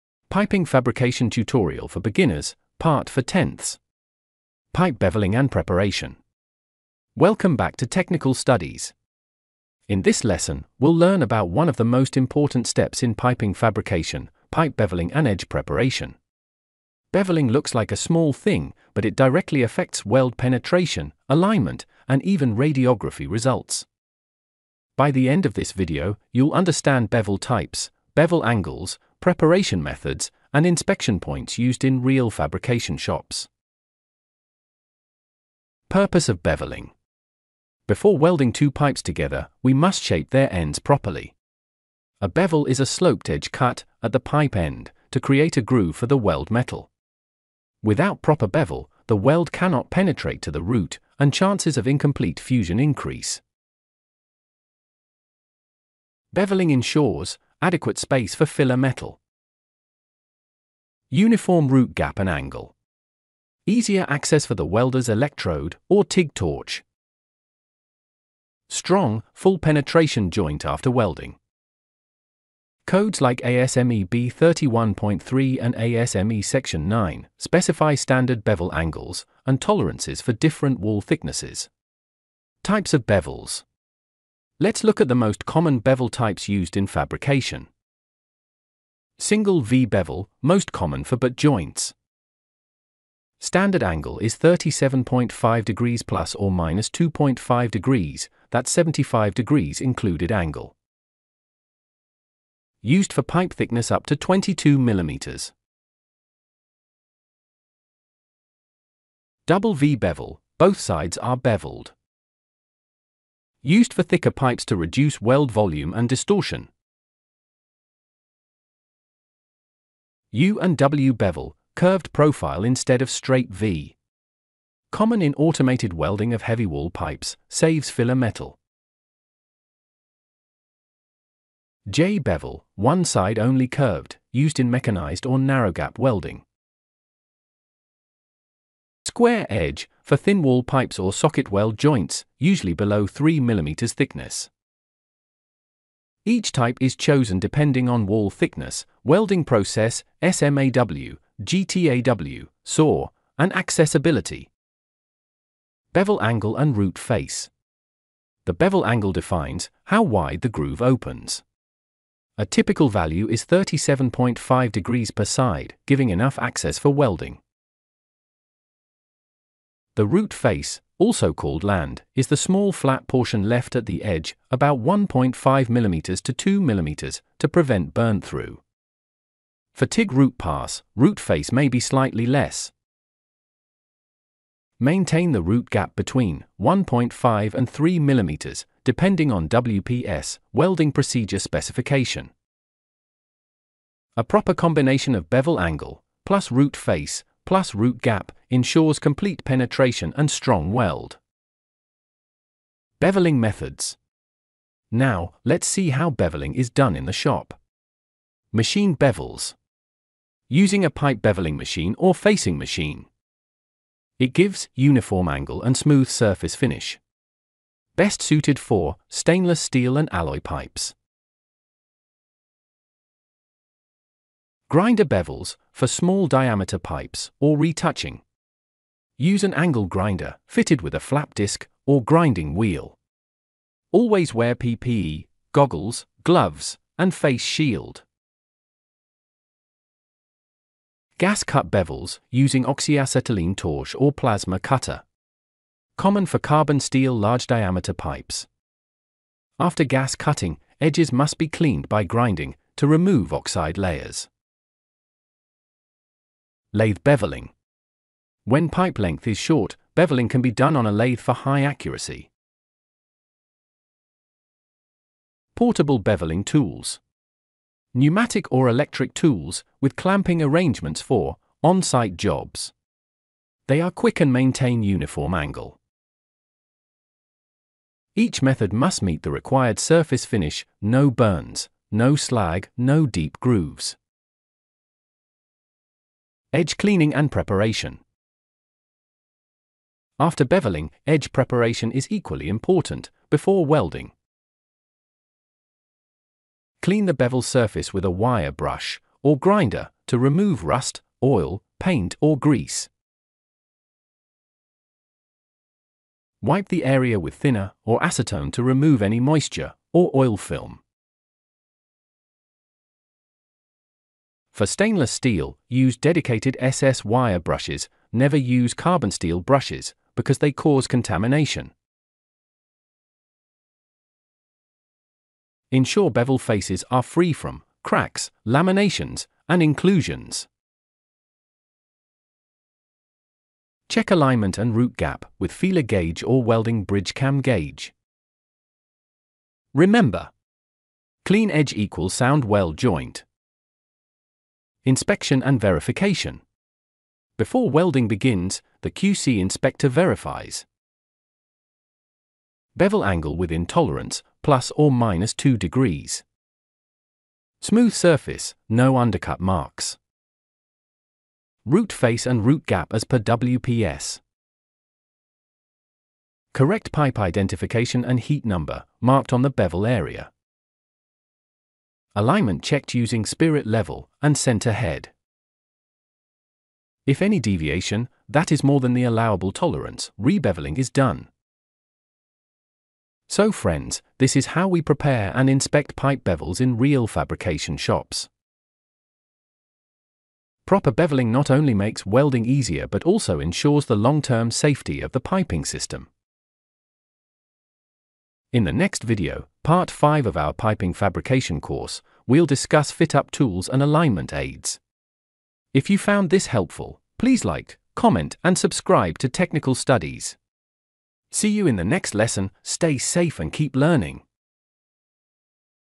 Introduction to Piping Fabrication – Part 4/10 | Pipe Beveling & Preparation Explained.
In this video, we’ll learn the complete process of pipe beveling and preparation before welding.
You’ll understand why beveling is required, how to maintain the correct bevel angle and root face, and the different tools used for manual, mechanical, and machine beveling.
This step is very important to ensure proper weld penetration and joint quality in piping fabrication.